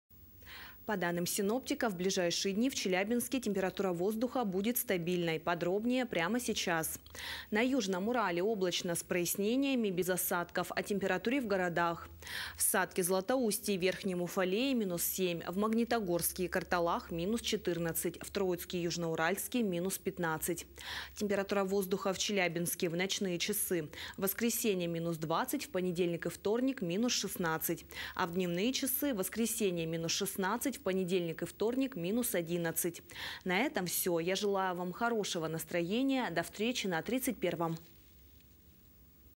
Погода от Сабрины Максимовской на 23, 24, 25января
Данные на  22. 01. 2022

В Челябинске пасмурно, осадков не ожидается.
Ветер, слабый, юго-восточный 1-2 м/с.
Температура воздуха  ночью -21 -22 днем  -14 -19.
Атмосферное давление будет повышенным и составит 752мм.рт.ст.